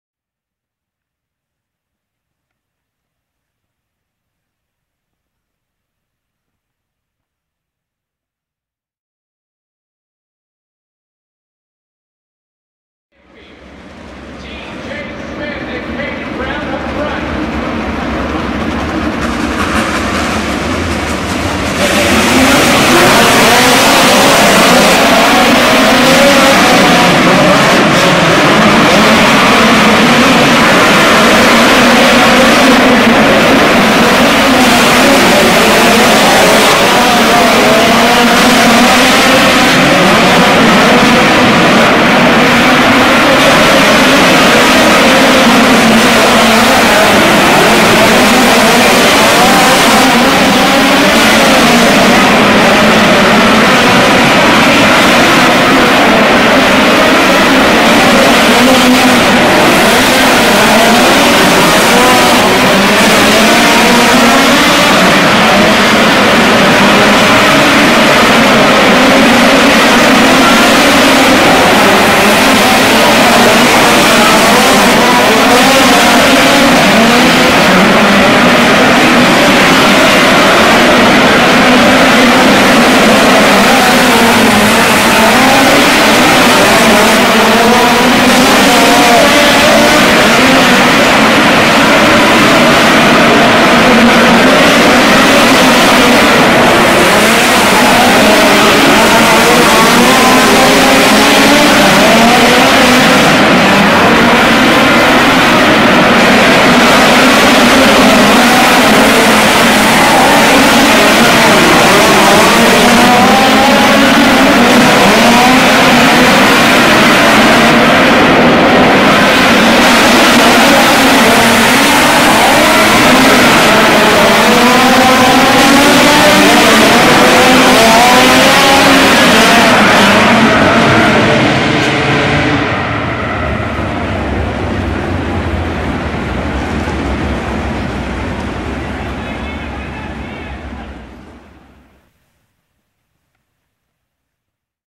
2020 Chili Bowl - Tuesday Heat 9
ChiliBowl2020 Tuesday Heat 9 (8 Laps): 1. 11S-TJ Smith[1]; 2. 11K-Gage Rucker[3]; 3. 97W-Kaidon Brown[2]; 4. 81X-Dillon Welch[7]; 5. 7U-Kyle Jones[5]; 6. F5-Ray Allen Kulhanek[8]; 7. 17F-Devin Camfield[6]; 8. 44S-Brandon Akau[4]